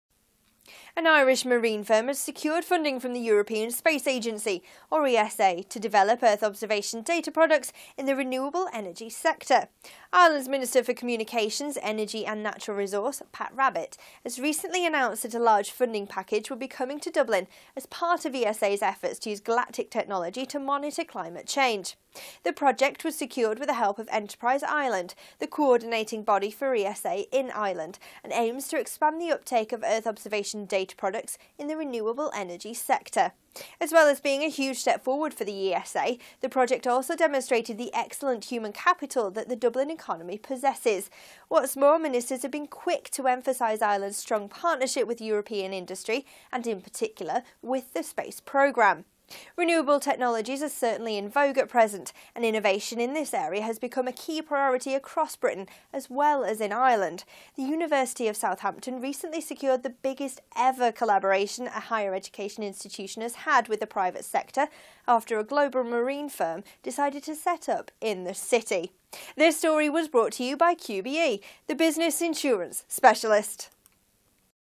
Dublin gets European Space Agency money
An Irish marine firm has secured European Space Agency funding to develop earth observation data products in the renewable energy sector.